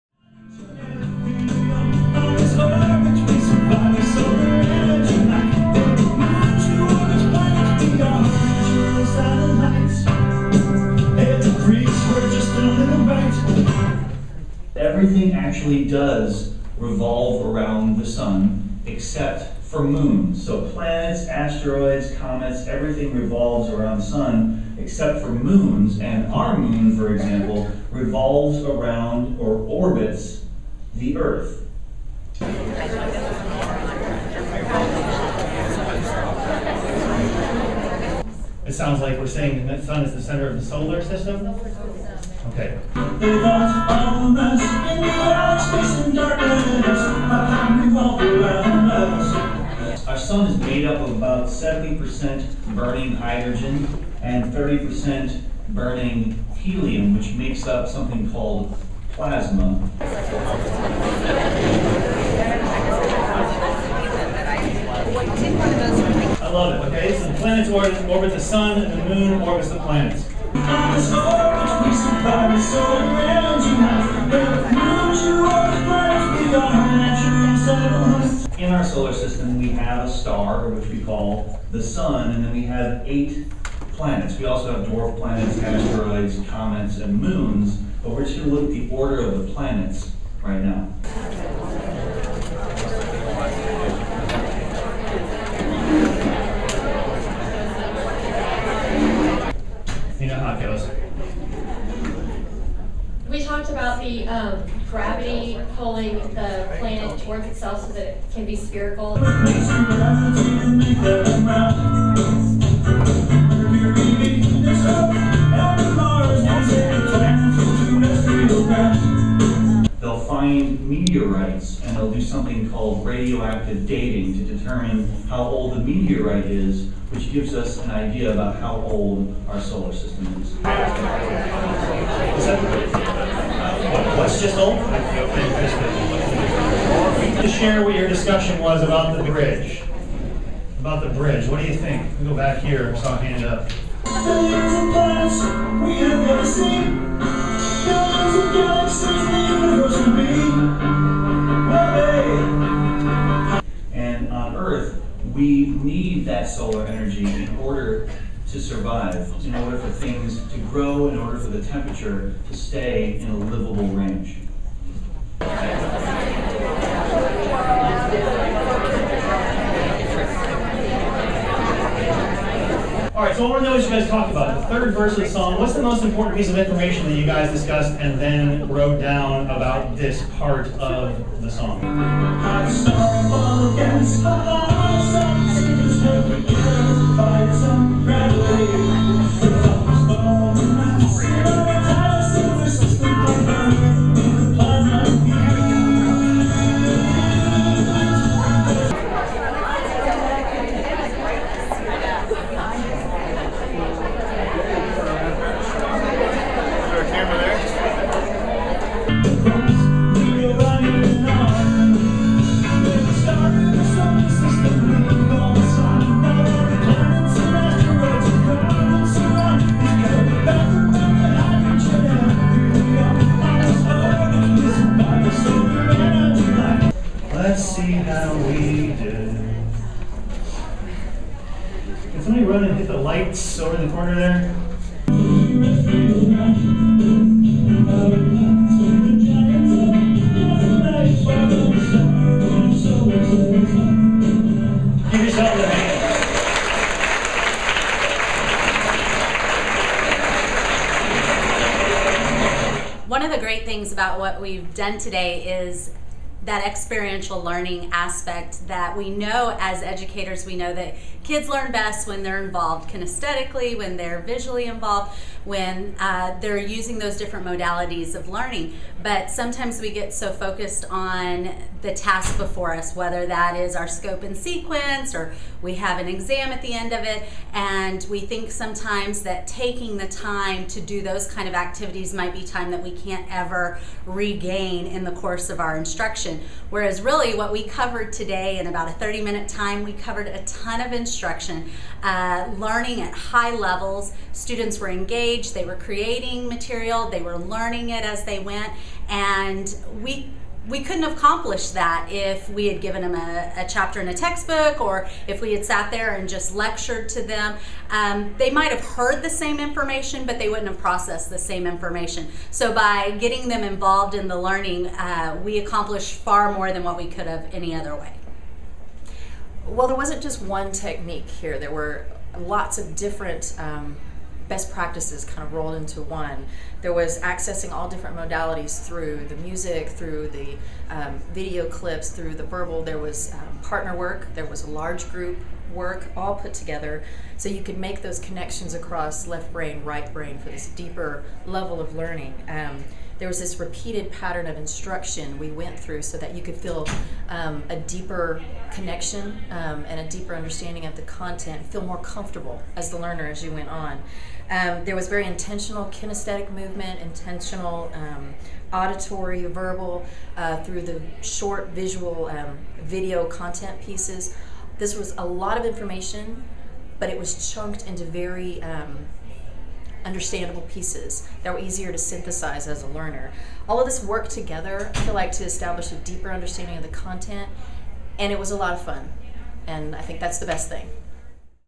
Montage Showing How I Teach Using Music & Movement (With Participant Responses) - Dr. Lodge McCammon
A group of STEM teachers from Texas get up and moving while they learn about the solar system during this Discovery Education & Texas ASCD event called Connecting the Dots with STEM Academy.

Video clips taken from a 60-minute lesson where I am challenging & engaging students using music, lecture videos, collaboration, discussion, movement, performance, publishing and reflection.